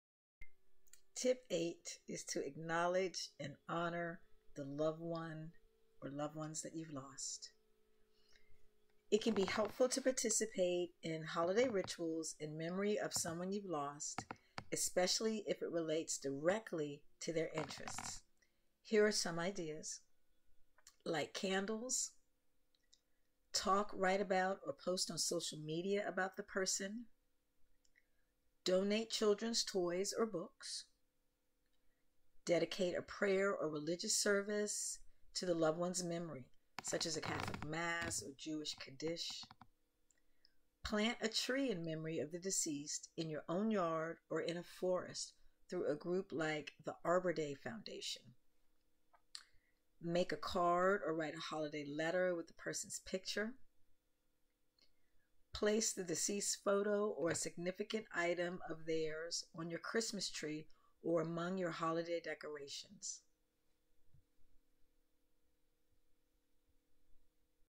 Tip 8 for Managing Grief During the Holidays
Just to let you know, the tip is to acknowledge and honor.

Thanks @aarp

For weekly support in your inbox: ⁠

To work one-on-one with me using mindfulness and Yoga to manage grief reach out: ⁠